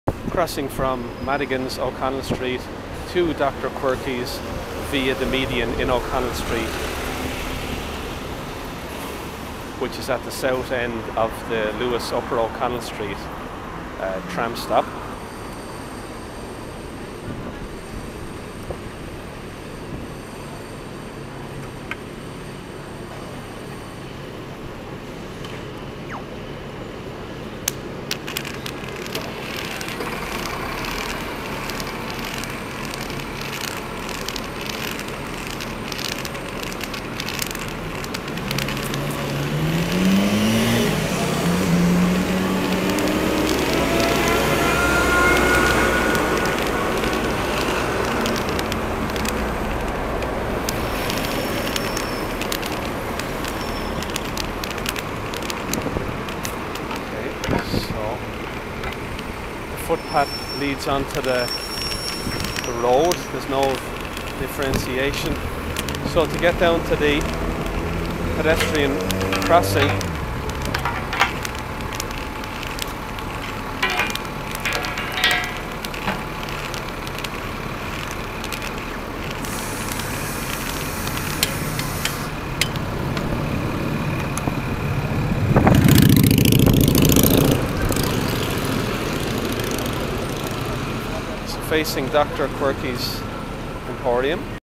Crossing from Madigans O'Connell Street to Dr. Quirkeys via median.
Bodycam video from person who is blind using a long cane navigating from pedestrian crossing at Madigans, O'Connell Street to Dr. Quirkeys via the median. Luas Cross City have built the footpath on the median with no kerb beside the track and is level with the track putting people who are blind or visually impaired in danger. A pedestrian safety railing needs to be installed from the cuordroy paving at the south end of the Upper O'Connell Street Luas stop as far as the pedestrian crossing facing Dr. Quirkey's to protect people who are blind or visually impaired from ending up on the track/road.